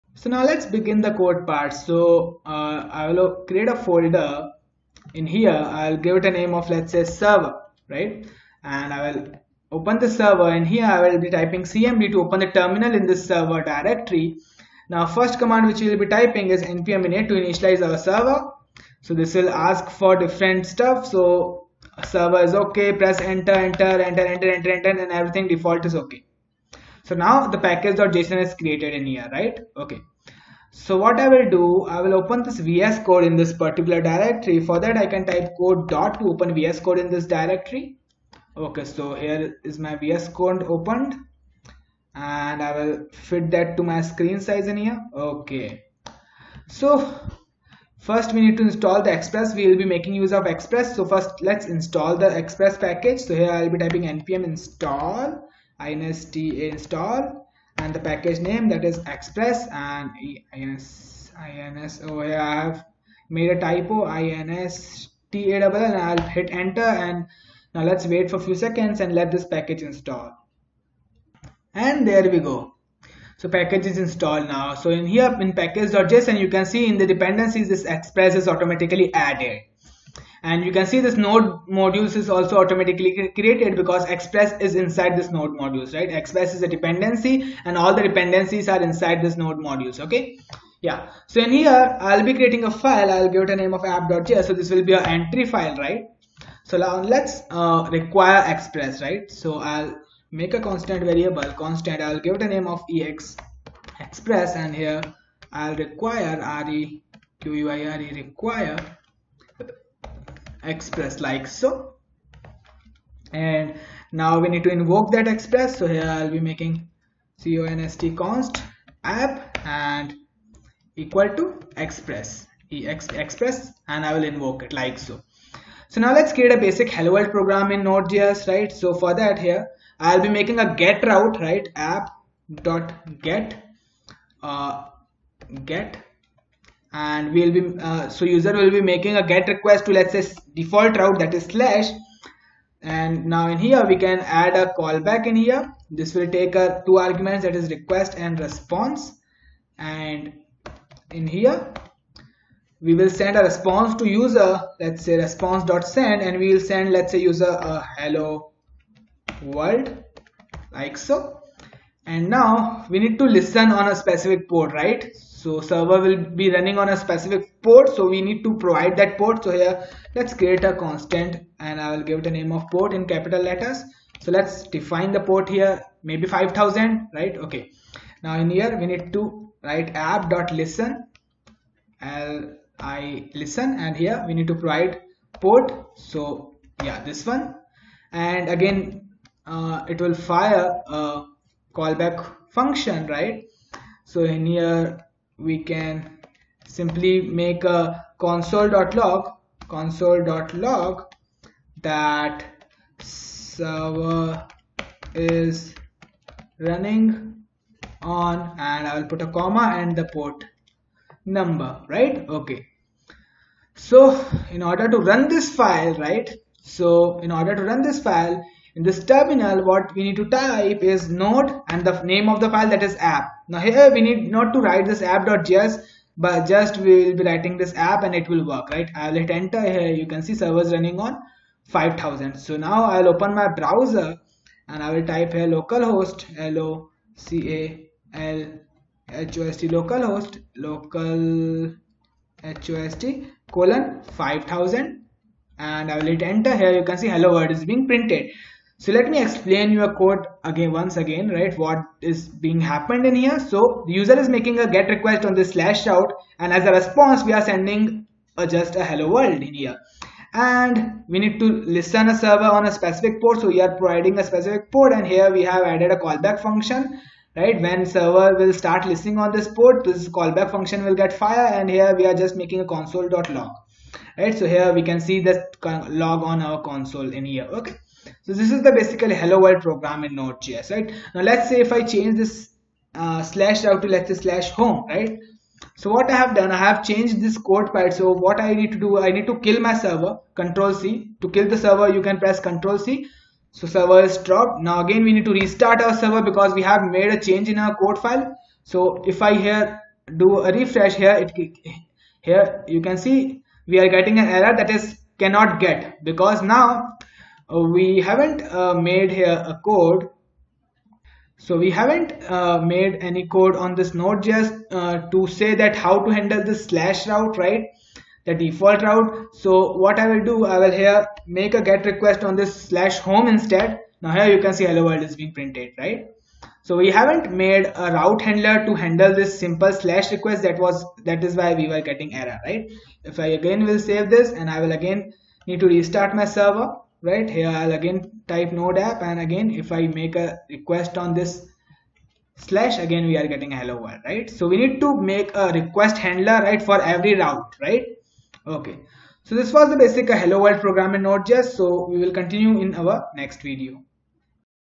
#1 - Hello world in node js | Instagram clone | MERN Stack tutorial
In this video we will create our first node js server MERN stack tutorial in hindi